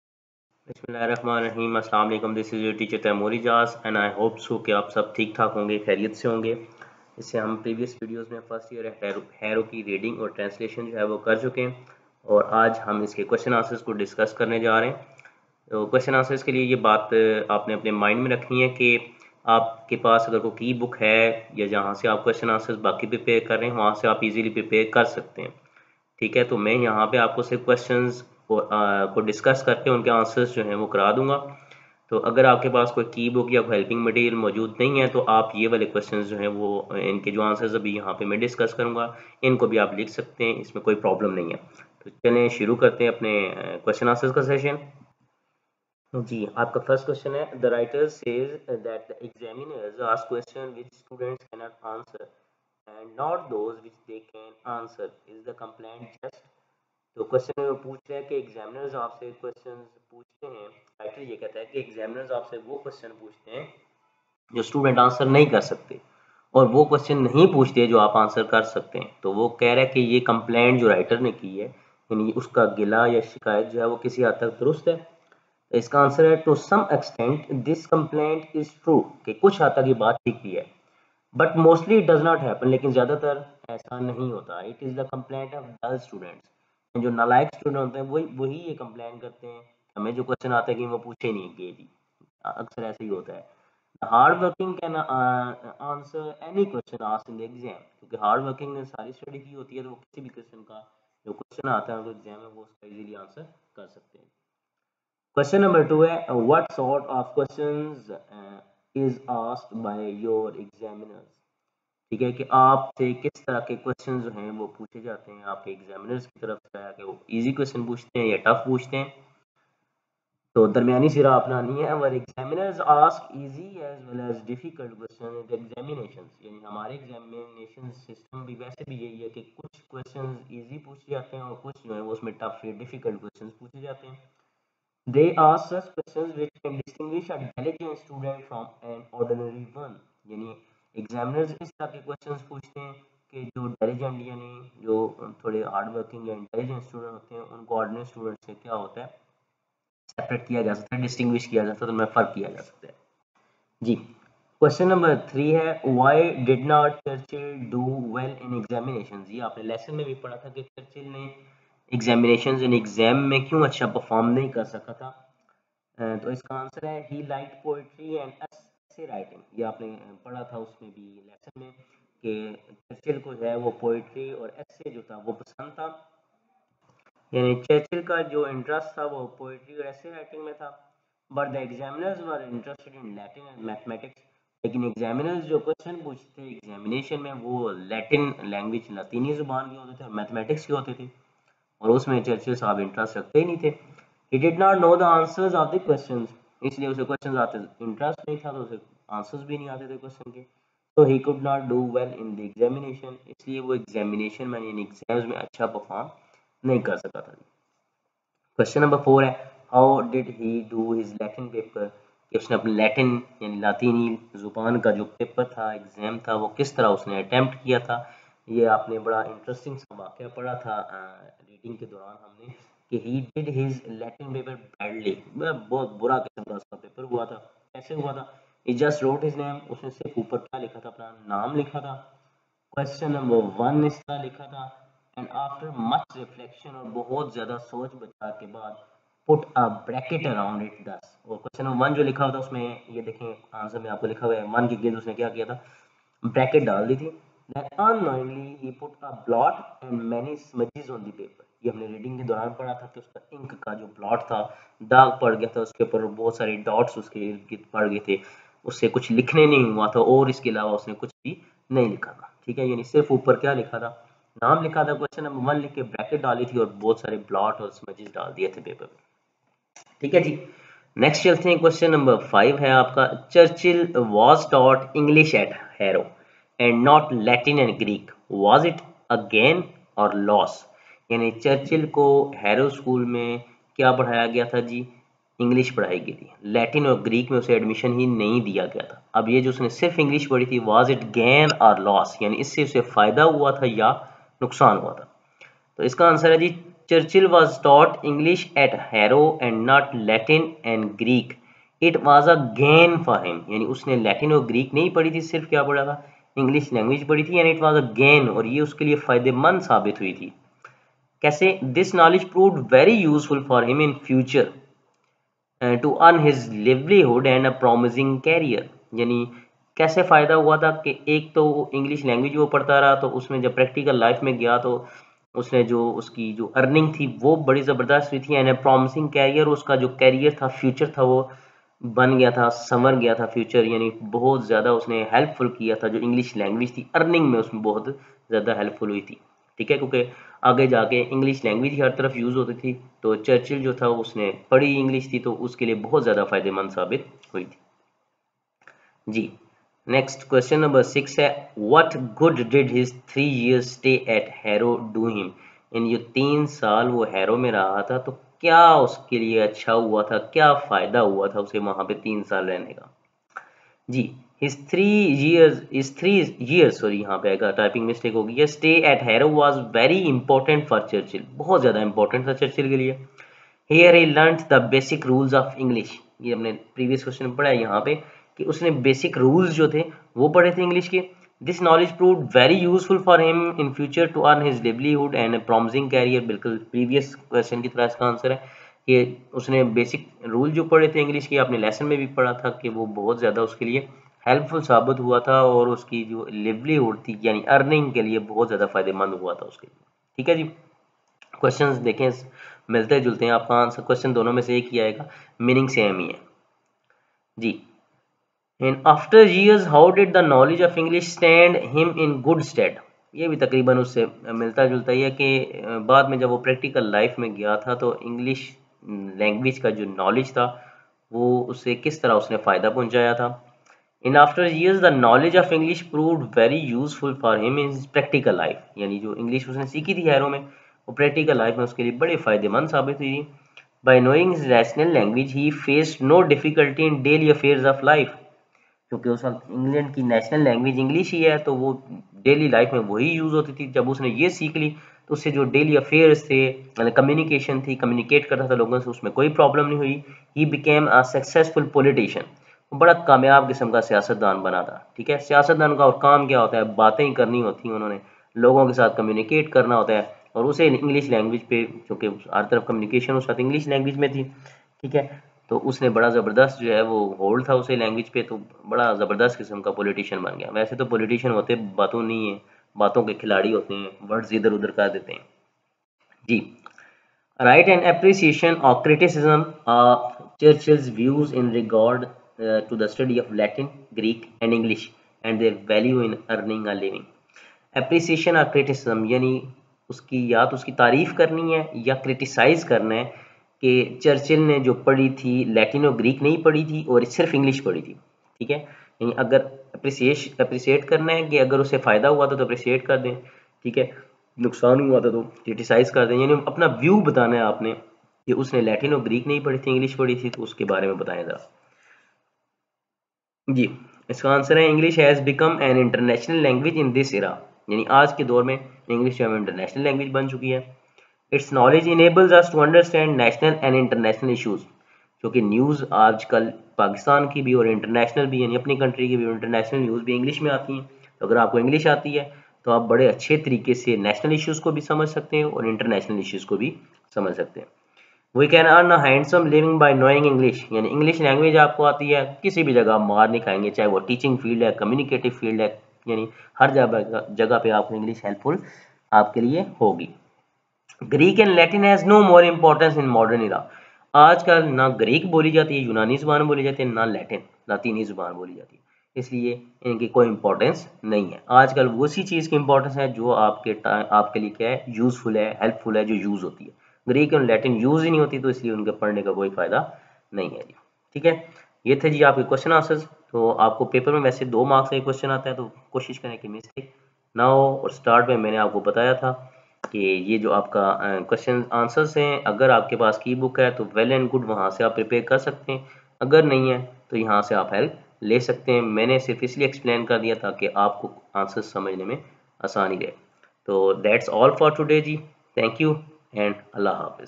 2nd Year English|| Chapter 11 " First Year at Harrow|| Important questions and answers explained
2nd Year English
Chapter 11
First Year at Harrow
Important questions and answers explained by Mr. Taimoor Ijaz Qureshi